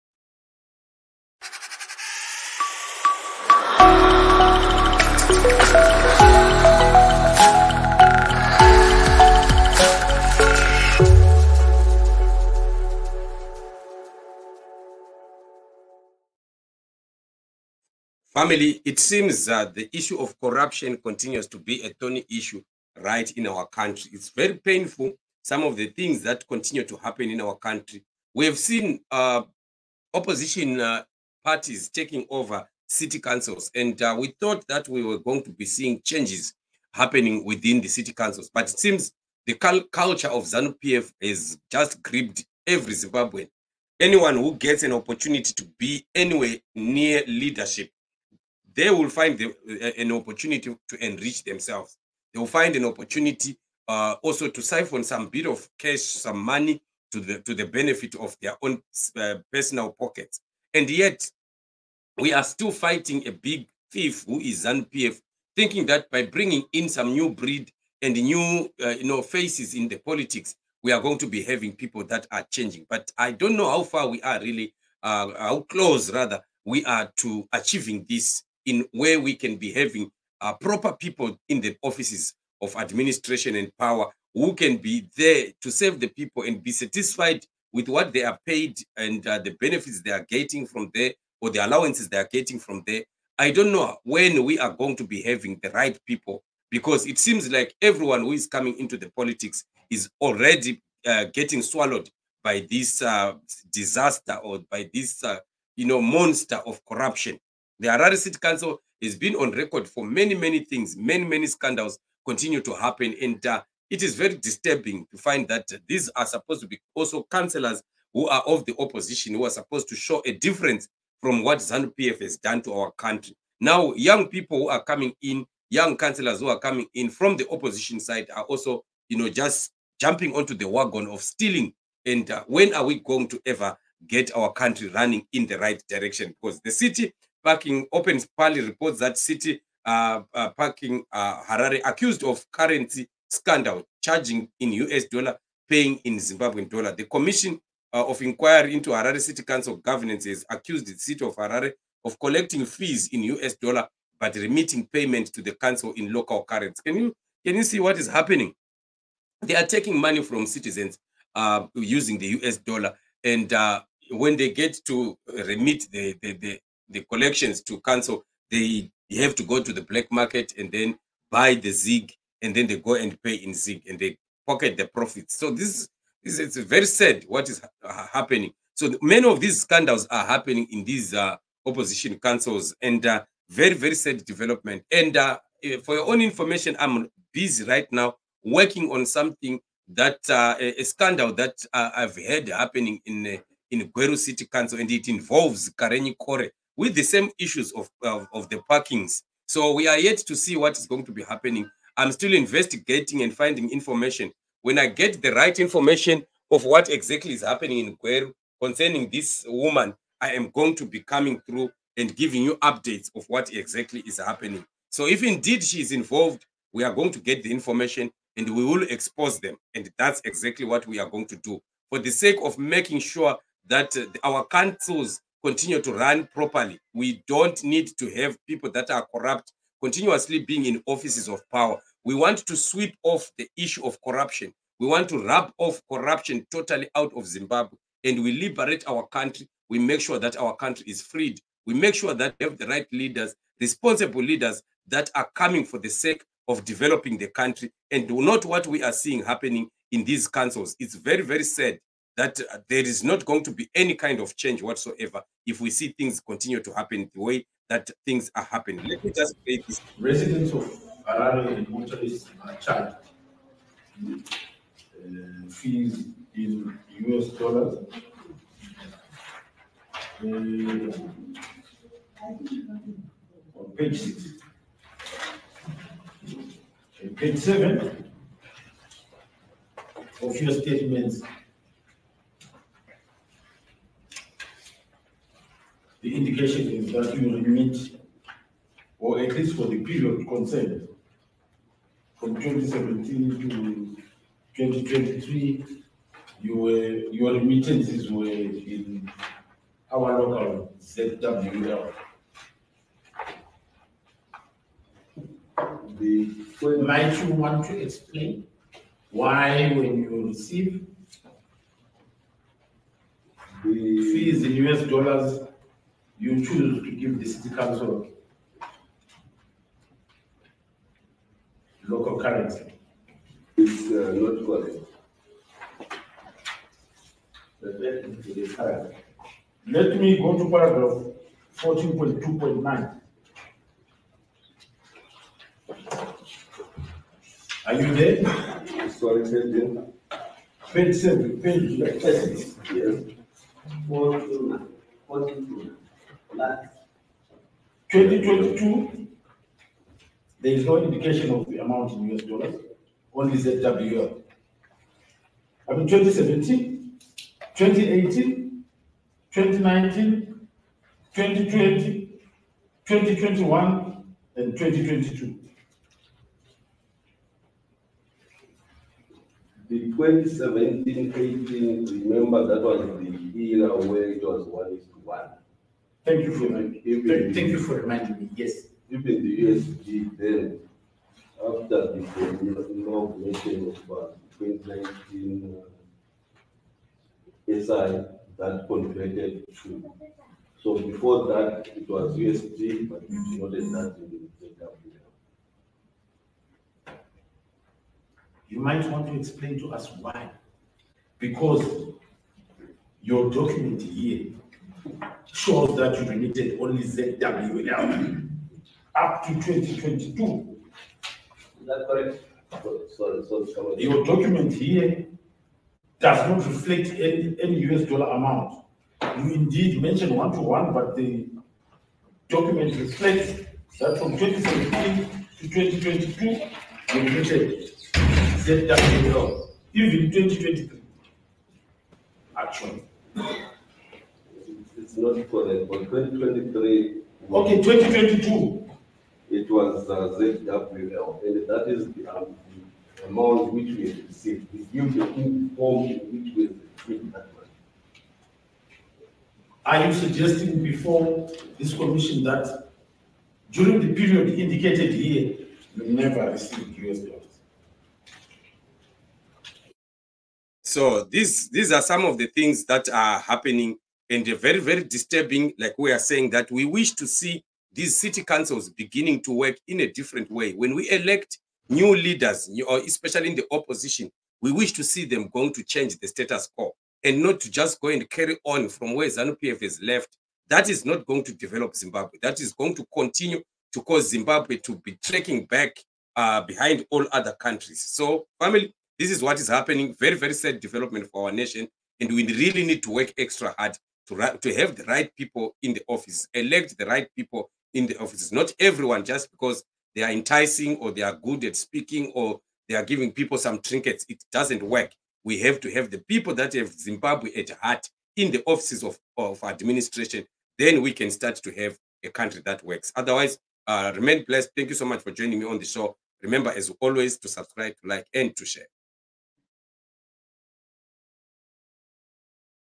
HARARE CITY COUNCIL PARKING IN A REVENUE SCANDAL
THE HARARE CITY COUNCIL DEPARTMENT OF PARKING HAS BEEN CAUGHT UP IN A PARKING REVENUE SCANDAL WHERE THRY CHARGE ZIMBABWEAN CITIZENS IN US DOLLAR YET THRY REMIT FUNDS TO COUNCIL IN LOCAL ZIG.